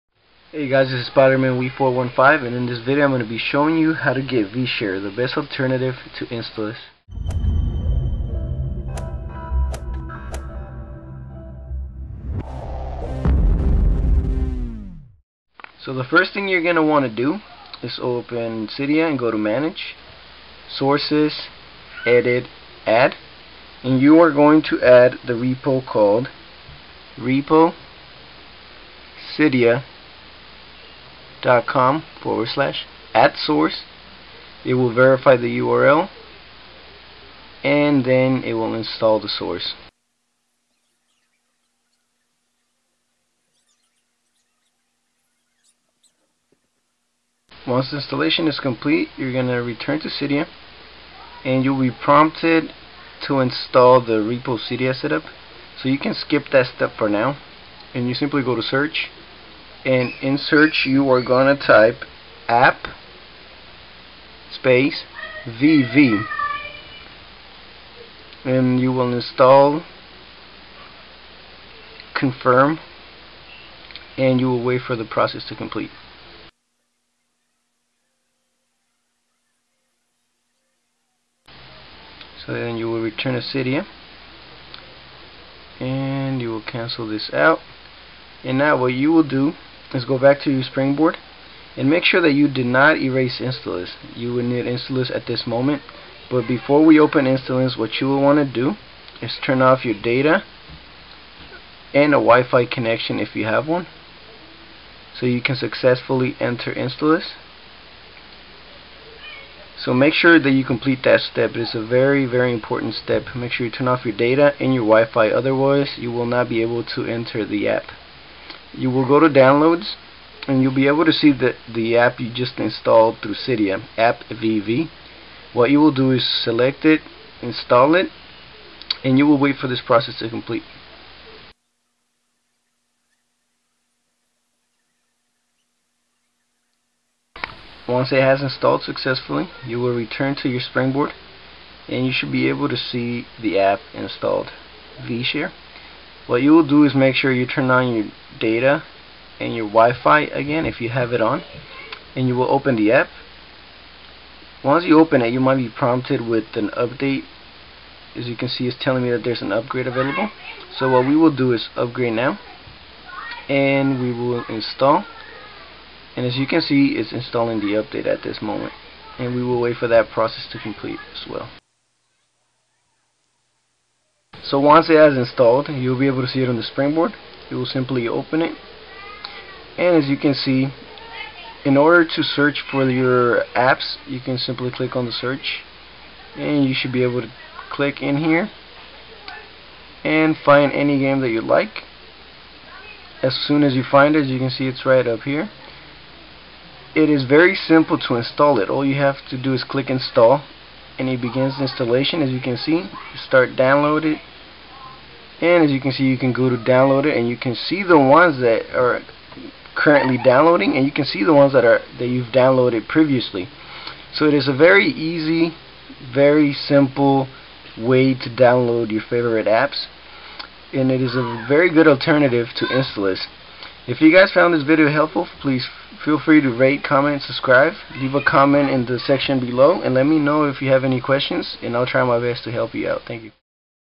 vShare - Best Installous Alternative 5.0/6.0/7.0
***New Video***

INSTALL APPSYNC 7 FOR iOS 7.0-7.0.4
1. *Note, if you do not have Installous you can restart your springboard after the installation from Cydia is complete. This, if successful, will install vShare on your springboard.*

2. **If you can't get vShare to work you can try installing it by using the following guide**

Since Installous shutdown many alternatives have come up. The best being vShare; with a simple user interface and well designed app.

In this video I will show you how to get vShare simply follow the steps in the video.
It is a very simple process.

**Please note that Installous may be required in order to complete the installation. This blog is for informational and entertainment purposes only.
In no way do I condone or approve of piracy.**
**Use the app at your own risk.**